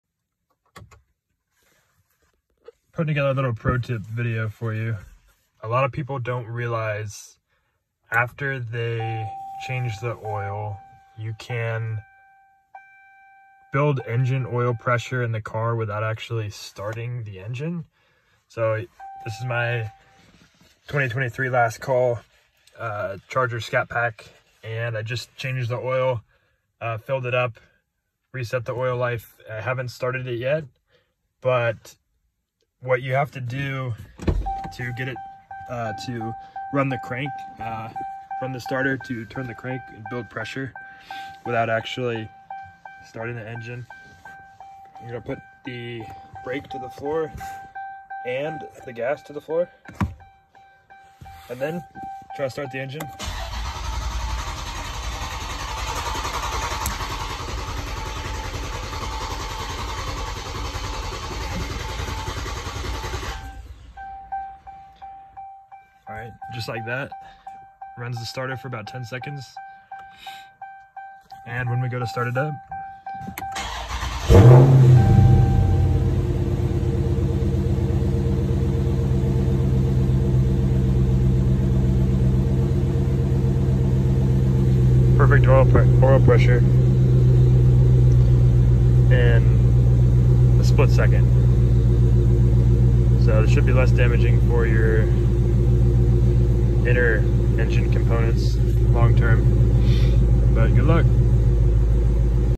Oil Change Pro Tip - build oil pressure without fully starting the car
Use google to see how you can do the same in your car.

Reason to do this:

When the engine is running, the exploding fuel/air pushing down on the piston puts dramatically more pressure on the bearing that just the engine turning over slowly from the starter. When the engine is running it will be turning at much more rpm than just the starter motor can provide.

It is a good idea to crank it without fuel and spark until it has oil pressure.